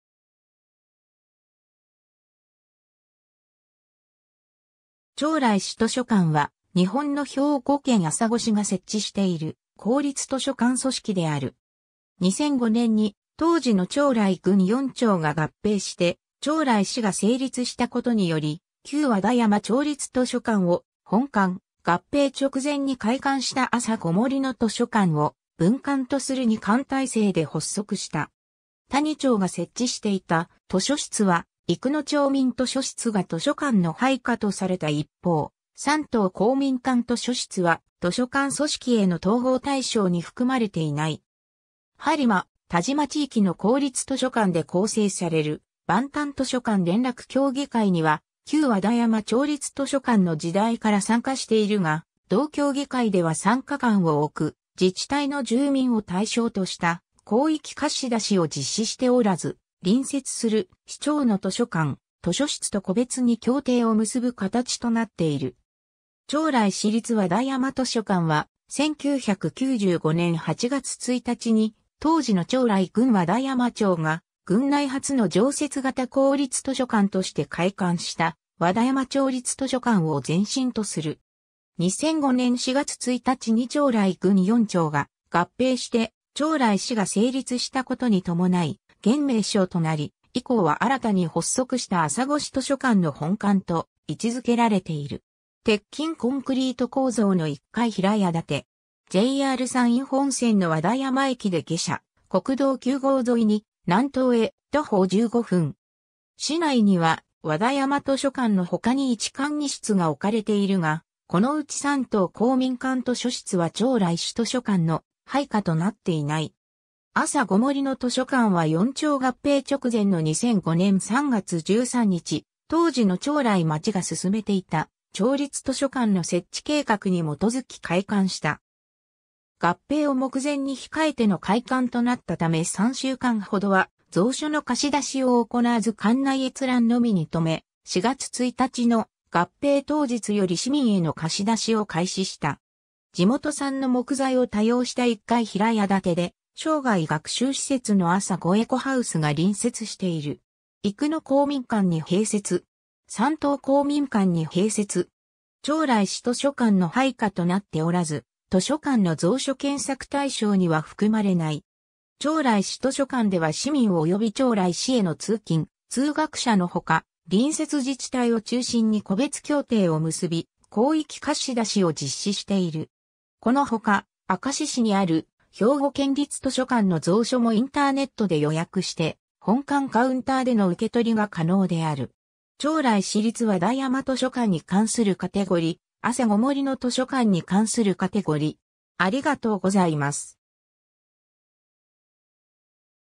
朝来市図書館
朝来市図書館（あさごしとしょかん）は、日本の兵庫県朝来市が設置している公立図書館組織である。
 2005年（平成17年）に当時の朝来郡4町（生野町、和田山町、山東町、朝来町）が合併して朝来市が成立したことにより、旧和田山町立図書館を本館、合併直前に開館したあさご森の図書館を分館とする2館体制で発足した。
他2町が設置していた図書室は生野町民図書室が図書館の配下とされた一方、山東公民館図書室は図書館組織への統合対象に含まれていない。
 播磨・但馬地域の公立図書館で構成される播但図書館連絡協議会には旧和田山町立図書館の時代から参加しているが、同協議会では参加館を置く自治体の住民を対象とした広域貸し出しを実施しておらず、隣接する市町の図書館・図書室と個別に協定を結ぶ形となっている。
 朝来市立和田山図書館（あさごしりつわだやまとしょかん）は、1995年（平成7年）8月1日に当時の朝来郡和田山町が郡内初の常設型公立図書館として開館した和田山町立図書館を前身とする。
2005年（平成17年）4月1日に朝来郡4町が合併して朝来市が成立したことに伴い現名称となり、以降は新たに発足した朝来市図書館の本館と位置付けられている。
 鉄筋コンクリート構造の1階平屋建て。
 JR山陰本線の和田山駅で下車、国道9号沿いに南東へ徒歩15分。
 市内には和田山図書館の他に1館2室が置かれているが、このうち山東公民館図書室は朝来市図書館の配下となっていない。
 あさご森の図書館（あさごもりのとしょかん）は4町合併直前の2005年（平成17年）3月13日、当時の朝来町が進めていた町立図書館の設置計画に基づき開館した。
合併を目前に控えての開館となったため3週間ほどは蔵書の貸し出しを行わず館内閲覧のみに留め、4月1日の合併当日より市民への貸し出しを開始した。
 地元産の木材を多用した1階平屋建てで、生涯学習施設のあさごエコハウスが隣接している。
 生野公民館（朝来市役所生野支所内）に併設。
 山東公民館に併設。
朝来市図書館の配下となっておらず、図書館の蔵書検索対象には含まれない。
 朝来市図書館では市民および朝来市への通勤・通学者の他、隣接自治体を中心に個別協定を結び広域貸し出しを実施している。
 この他、明石市にある兵庫県立図書館の蔵書もインターネットで予約して本館カウンターでの受け取りが可能である（県立図書館の利用券は別途必要）。
 朝来市立和田山図書館に関するカテゴリ: あさご森の図書館に関するカテゴリ: